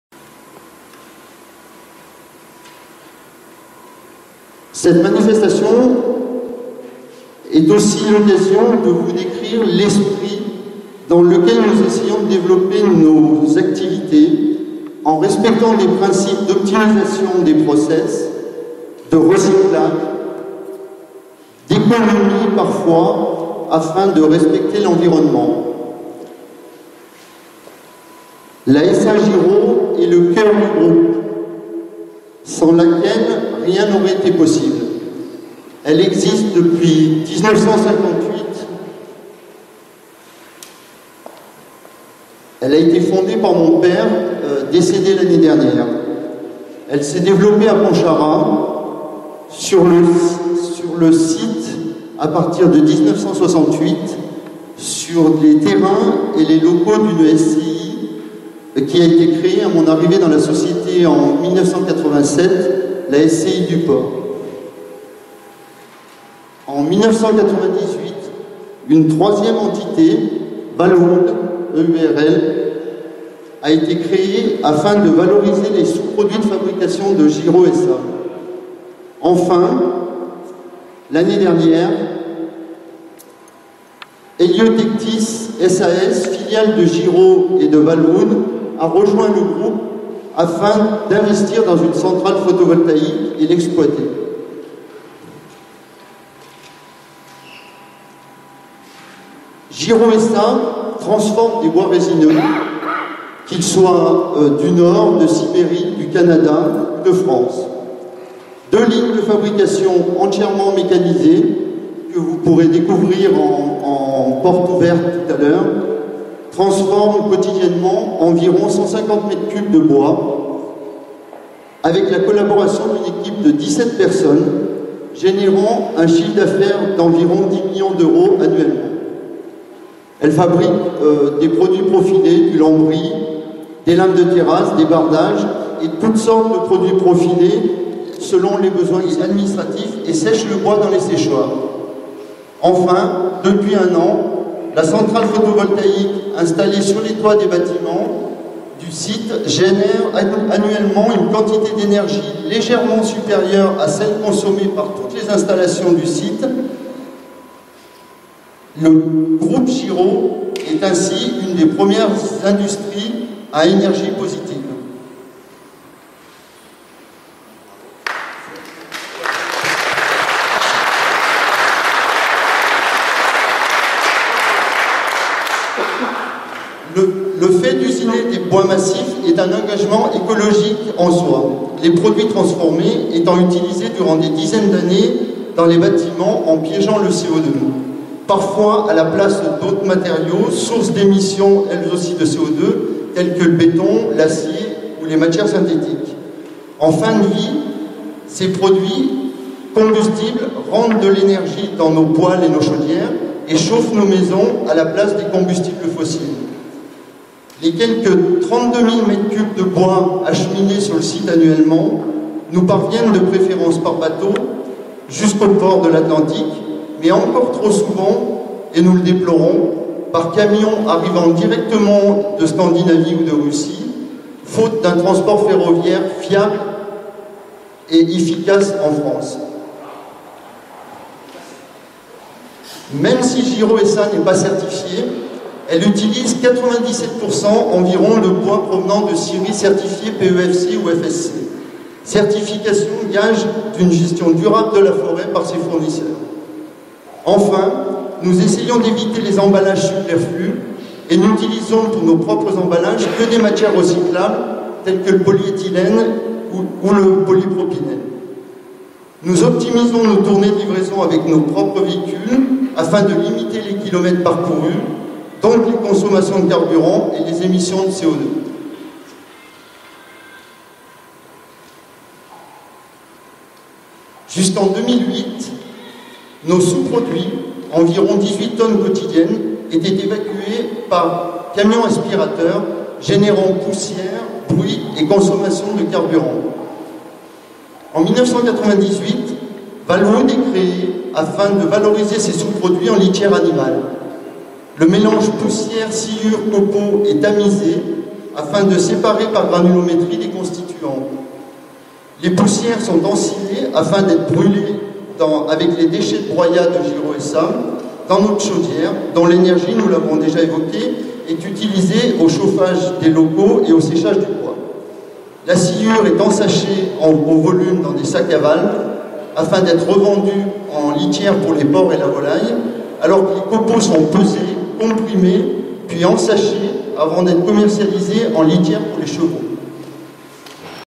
présentation Mr Girod - Girod SA
inauguration de la centrale photovoltaïque a Pontcharra chez Girod S.A installée et gérée par Cap sud Holding  le 21 Septembre 2012